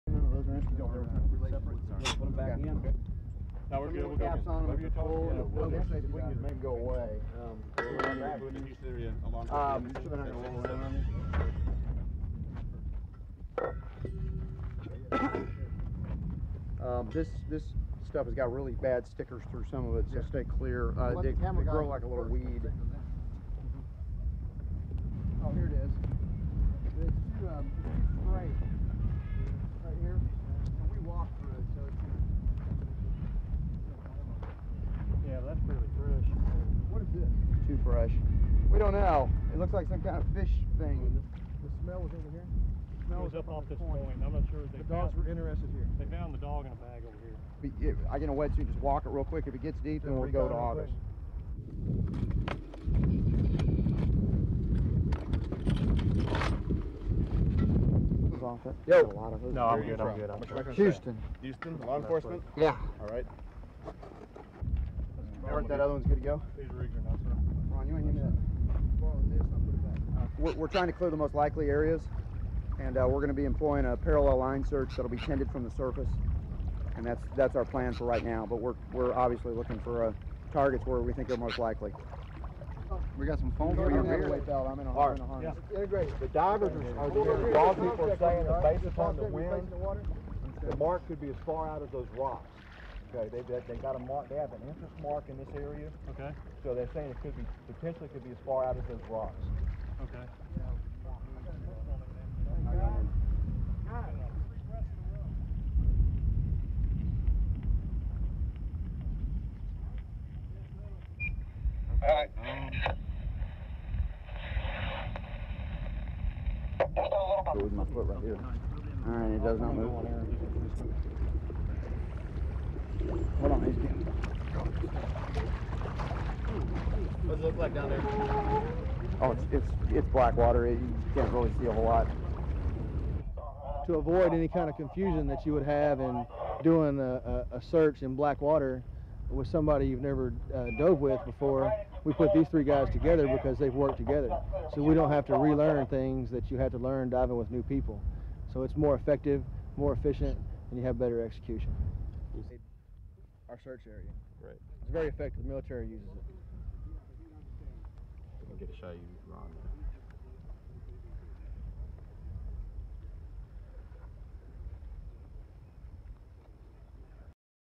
Latest on search for missing US teenager
1. Search Team arranging oxygen tanks, tilt down to tanks
2. Searchers pointing in distance
3. Searchers moving toward water and examining tracks
4. Searchers examining water
5. Searcher pointing
6. Man with equipment in trunk of car
7. Various of search team arranging equipment, oxygen tanks and wet suits
8. SOUNDBITE: (English) Mark Jansan, Search Team Member:
"We're trying to clear the most likely areas. We're employing a parallel line search that'll be 10 feet from the surface. That's our plan for now. Obviously we're looking for targets where we think it's more likely."
9. Diver, pointing to ground then sitting next to older diver, tilt up to team member
10. Various of searchers in water
11. SOUNDBITE: (English) Ron Gilbert, Search team member:
"To avoid any kind of confusion that you would have in doing a search in black water with somebody you've never dove with before, we put these three guys together because they've worked together; so you don't have to re-learn things that you have to learn when diving with new people. So it's more effective, more efficient and you have better execution."
12. Pan from Ron Gilbert to document and then up to member of search team

STORYLINE:

A team of volunteers from the Houston, Texas area searched Aruba on Monday for any clues leading to the disappearance of American teenager Natalee Holloway.

The 24 member team searched the wetlands to assist the efforts of the FBI and the Aruban authorities.

Very deliberately, they are scanning the depths of waters and the shorelines using sophisticated equipment and highly trained personnel.

Texas EquuSearch, the organisers of the team, were asked to assist in the search by the family of the missing girl.

They are conducting their work around a lighthouse that was referenced by a suspect during the investigation.

The group is working in teams on the ground and in the water hoping to find clues.

The American teenager has been missing for more than three weeks now.

The search team is using sonar, trained dogs and diving equipment to discover the fate of Natalee Holloway.

Most of the team are members of law enforcement or emergency response teams that are in Aruba non-officially as civilian volunteers.

So far, there has been no sign of what happened to Natalee Holloway.